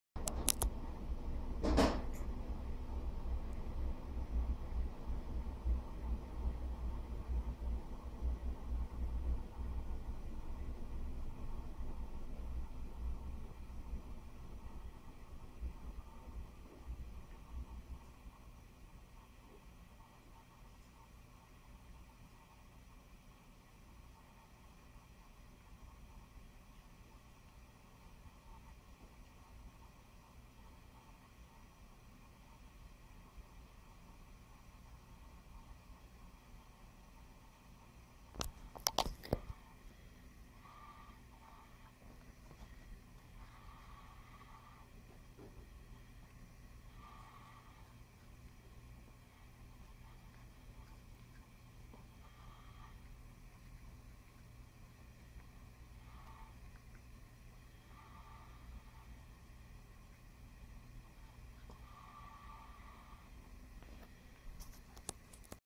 Cat eating clothes
OK so some of you have dogs that chew up everything, we have a cat. Jag is chewing up our clothes since he was a baby, We have lost several shirts, sucks and other clothing items to him. Once he ate half of my shorts. At first we thought we had a moth problem, but it was the cat! I just finally caught him doing it and he didn't even care I was sitting right there making a video... Crazy cat! lol